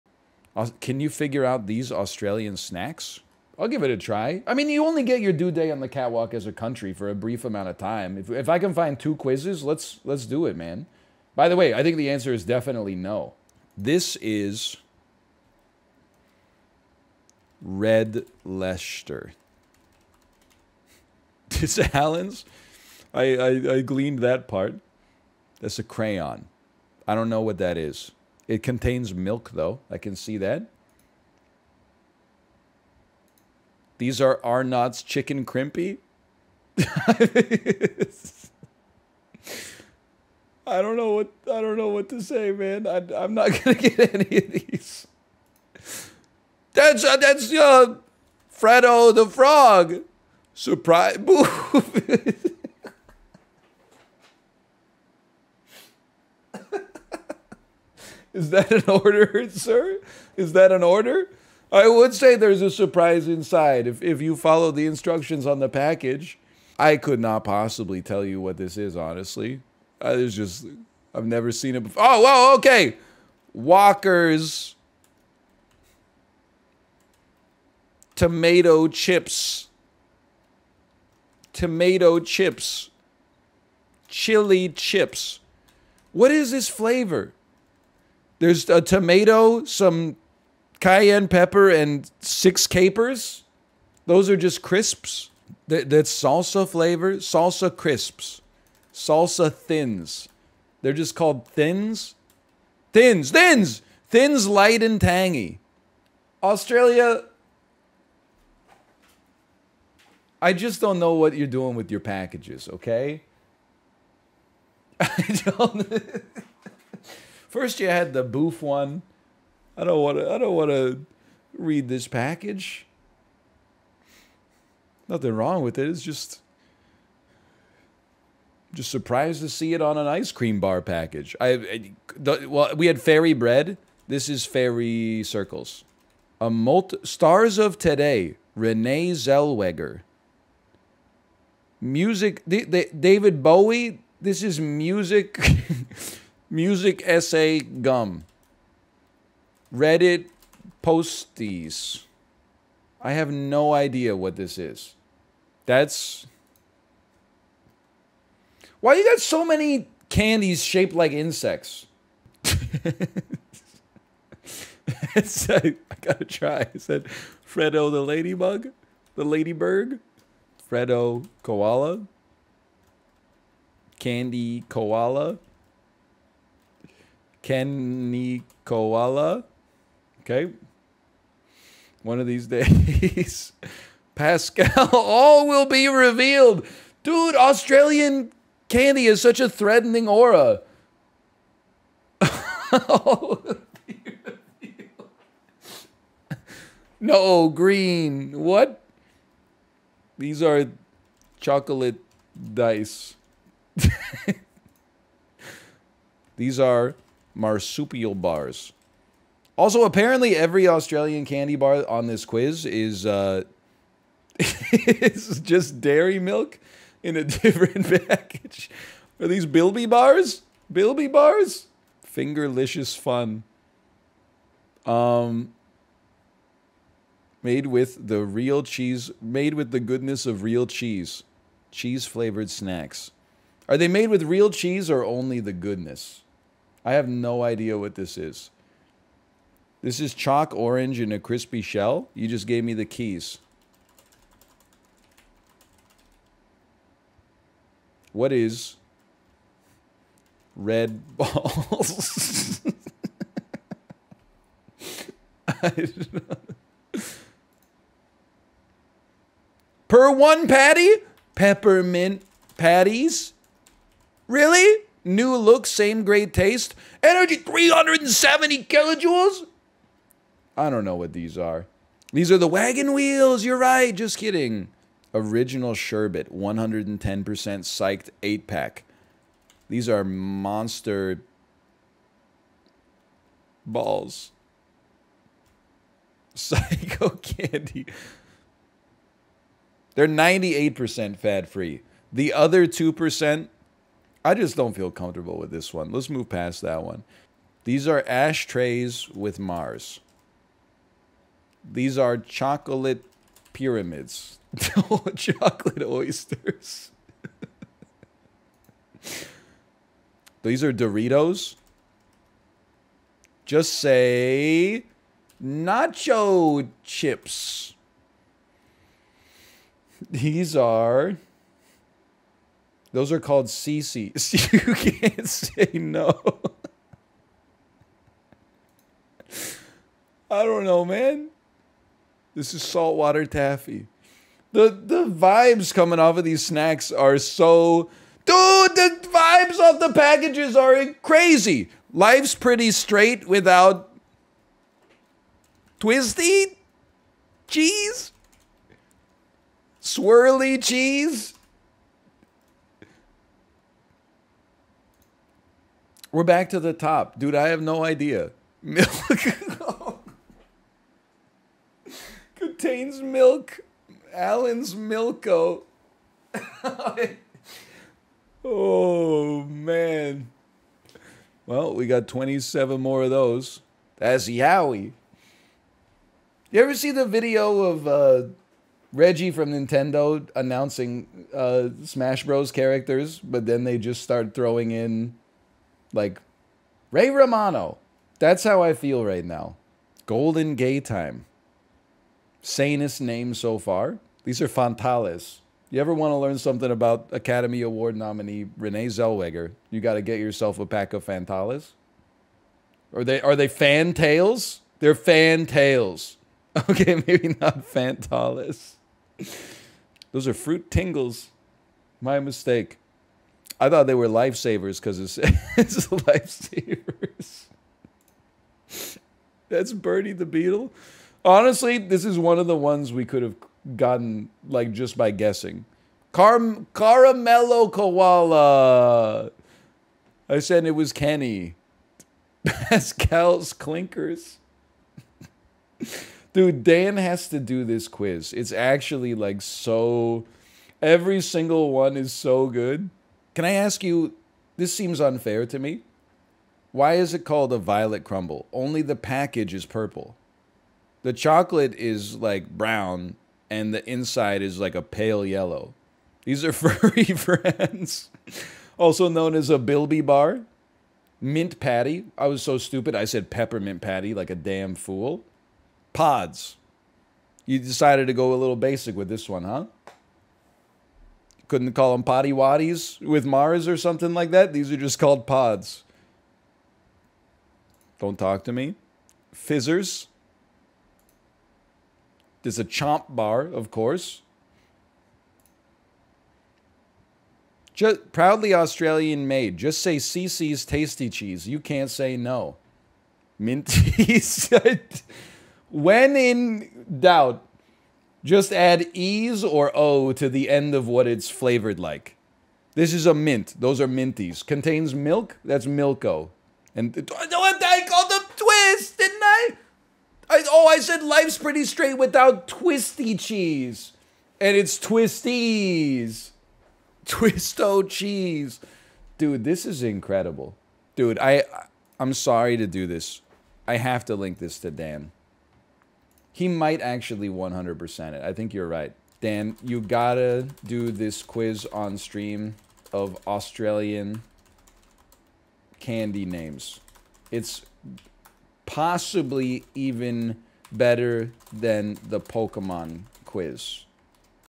Northernlion breaks down over Australian snack names
pods kinda goated though